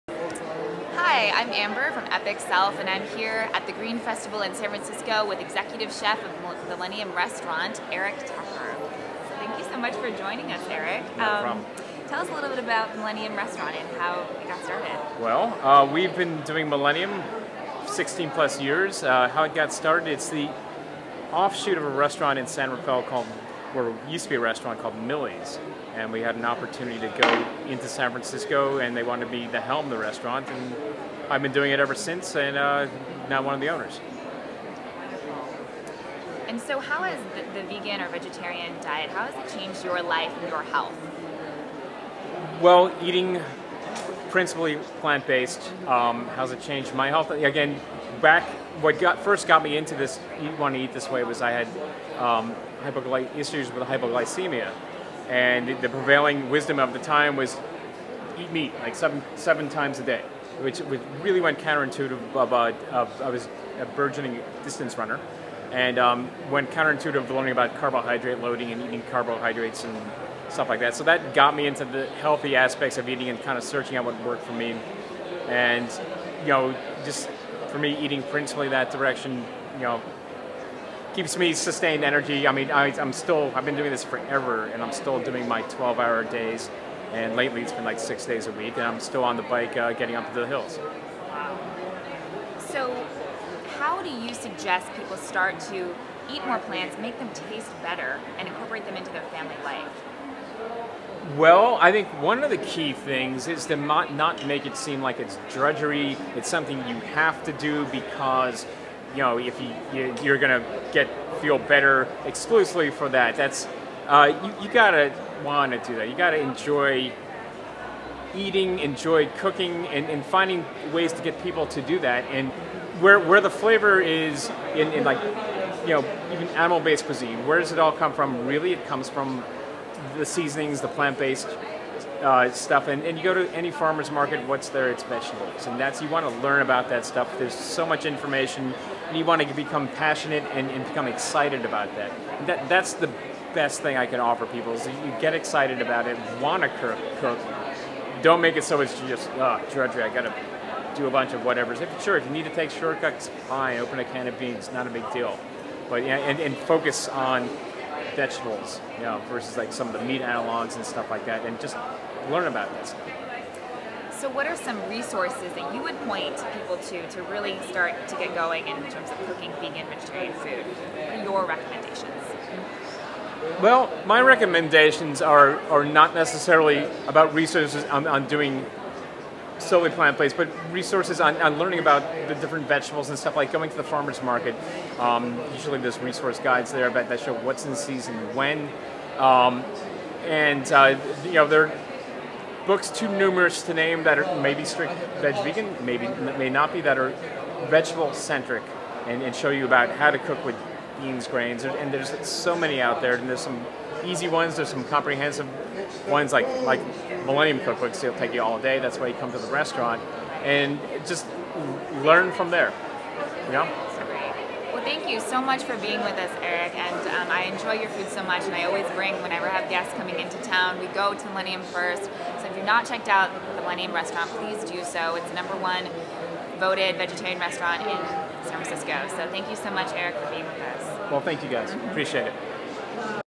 Exclusive Green Festival Interview: Executive Chef Eric Tucker- Millennium
Eric and I chatted back stage after the plant based nutrition discussion on main stage. His insight into sharing amazing veggie meals with the masses was a highlight of the nutrition presentation. Millennium is the #1 gourmet vegetarian restaurant in San Francisco after all. If it tastes good people will eat it!

Learn how a plant based diet transformed his life and fuels his passion for creating delicious food.